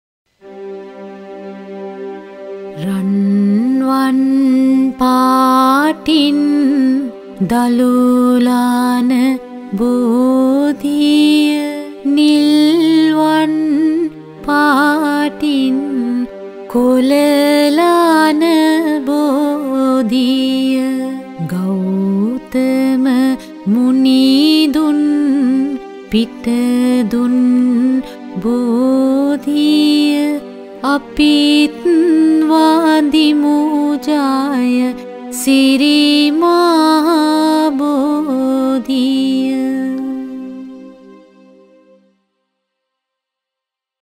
1000 Folk songs by Wasantha Madurangani - ජන ගී 1000ක් - Bathi Gee (බැති ගී)
1000 Sri Lankan Folk songs for the future by Wasantha Madurangani
වසන්තා මධුරංගනීගේ අනාගතයට ශ්‍රී ලාංකේය ජන ගී 1000ක්.